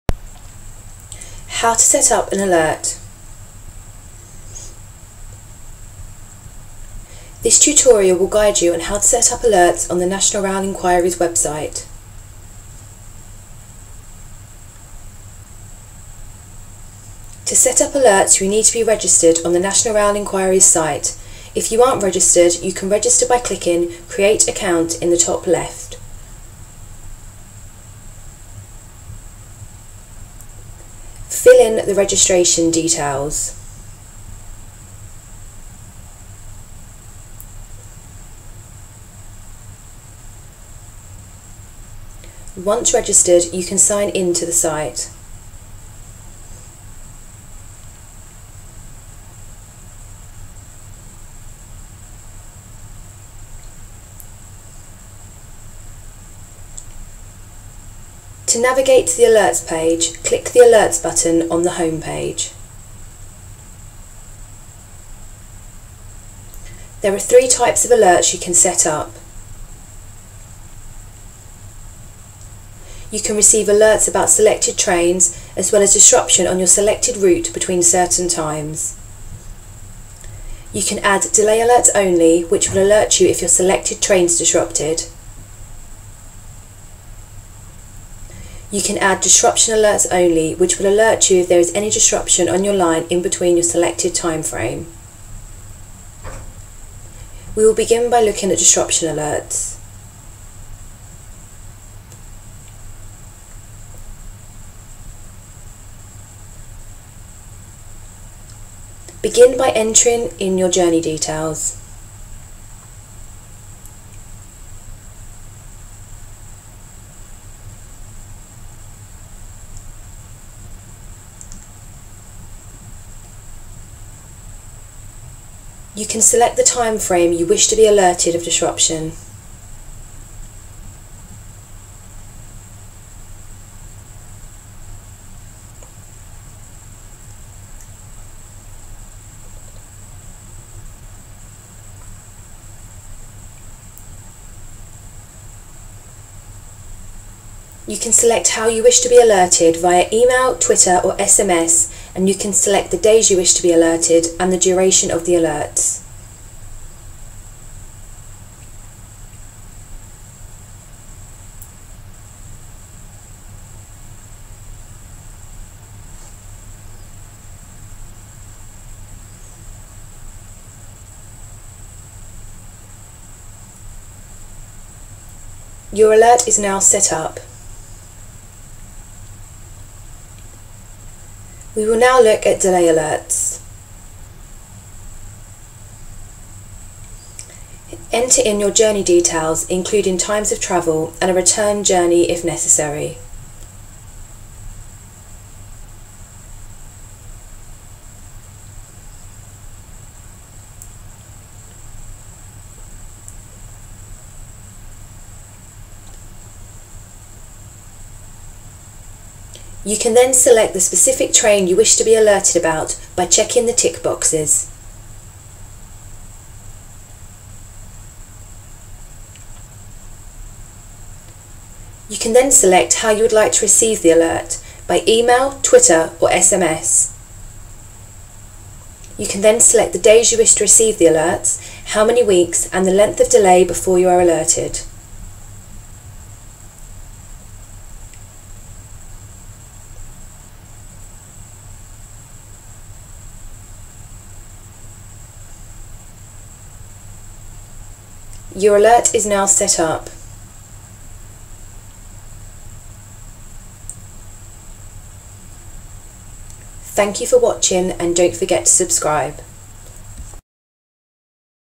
How to set up Alerts on the National Rail Enquiries Website video
This handy tutorial video shows you how to set up journey alerts using the National Rail Website. You can receive the alerts through Twitter DM, SMS or Email by registering on our Along with setting up alerts, you can also find the cheapest train tickets, train prices and buy train tickets.